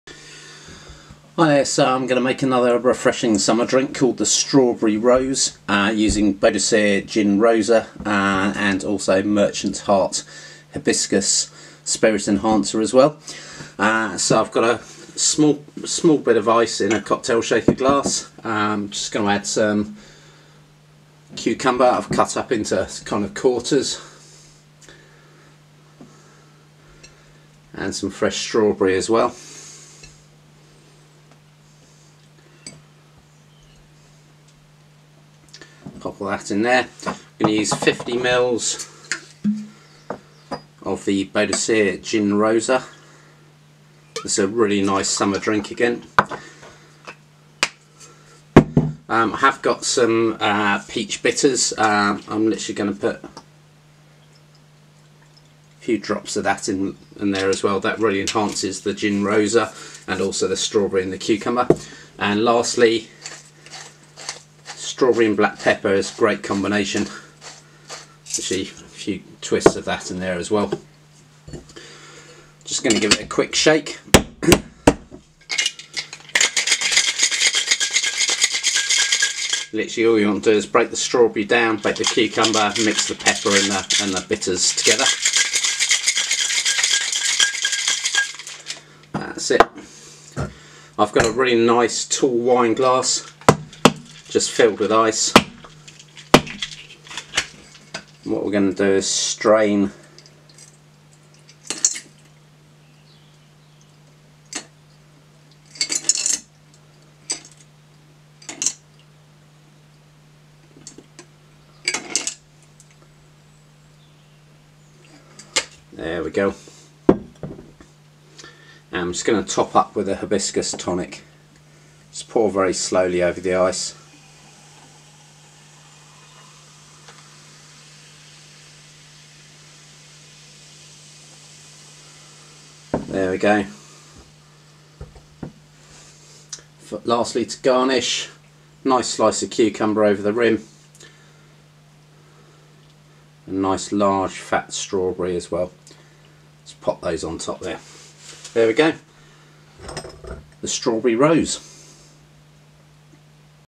Boadicea Gin Rosa 'Strawberry Rose' cocktail
Lockdown Cocktail: Boadicea Gin Rosa 'Strawberry Rose' cocktail

Under lockdown but still wish to enjoy your cocktails? Paul shows you how to create a delicious pink gin cocktail, full of Boadicea Gin Rosa Pink Gin, complemented with Merchant's Heart Hibiscus Spirit Enhancer and garnished with strawberry, cucumber and black pepper, delicious!

Paul is usually found running busy bars but like many of us, is currently in lockdown.

Paul Richardson - Deputy General Manager at Coaching Inns Group

How to make the Boadicea Gin Rosa 'Strawberry Rose' cocktail
- 50ml Boadicea Gin Rosa
- Black pepper
- Diced Strawberry
- Diced Cucumber
- Merchants Heart Hibiscus Spirit Enhancer
- Large Strawberry
- Slice of Cucumber
- Peach (or Angostura) bitters

1 - Add the Boadicea Gin Rosa to a cocktail shaker with ice.
2 - Add the diced strawberry and cucumber.
3 - Add 3 or 4 twists of ground black pepper.
4 - Add a few drops of bitters.
5 - Shake hard for about 30 seconds.
6 - Pour the contents of the cocktail shaker into the wine glass and then top up with ice.
7 - Pour Merchants Heart Hibiscus Spirit Enhancer over the top.
8 - Garnish the rim of the glass with a whole strawberry and a slice of cucumber.